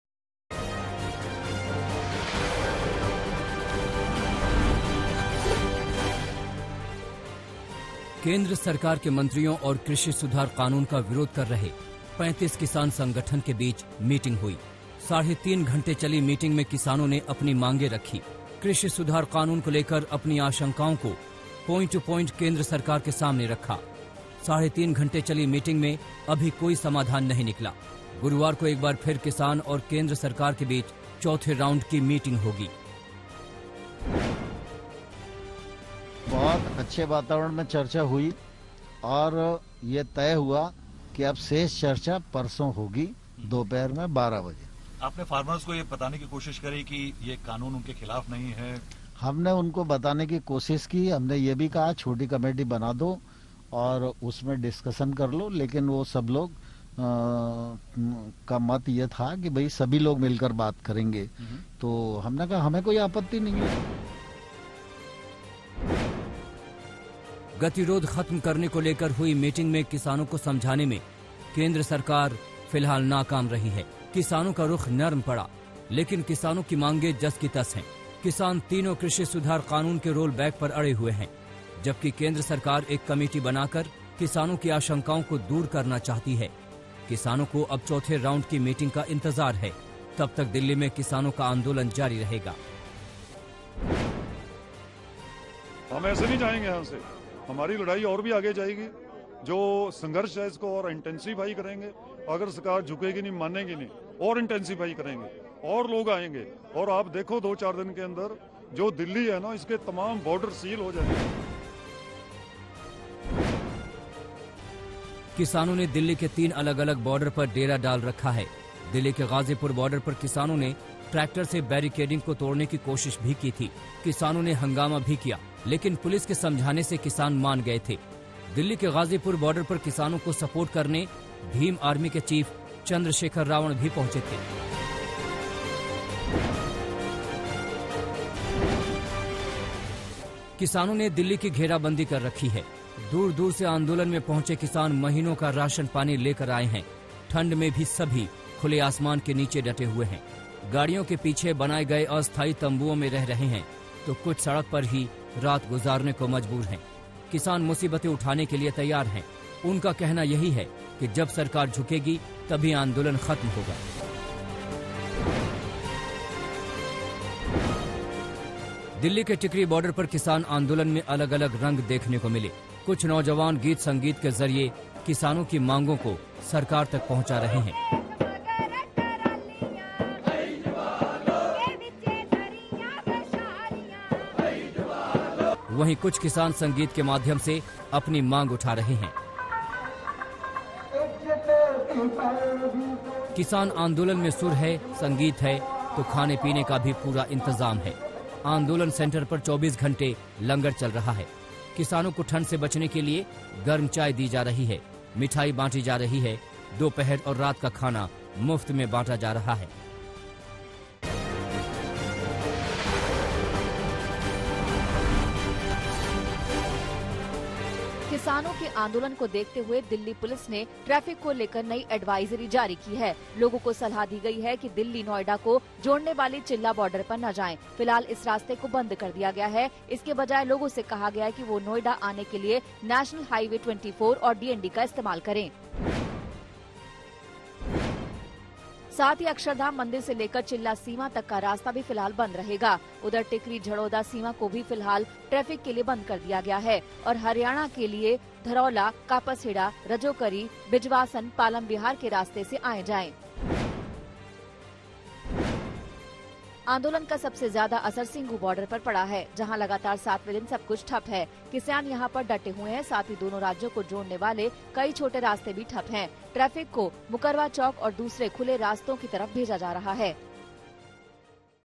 Farmers Protest नए कानून को रोल बैक करने पर अड़े किसान, नहीं निकला कोई नतीजा
केंद्र सरकार के मंत्रियों और कृषि सुधार कानून का विरोध कर रहे 35 किसान संगठन के बीच मीटिंग हुई. साढ़े तीन घंटे चली मीटिंग में किसानों ने अपनी मांगें रखी, कृषि सुधार कानून को लेकर अपनी आशंकाओं को प्वाइंट टू प्वाइंट केंद्र सरकार के सामने रखा. साढ़े तीन घंटे चली मीटिंग में अभी कोई समाधान नहीं निकला।गुरुवार को एक बार फिर किसान और केंद्र सरकार के बीच चौथे राउंड की मीटिंग होगी.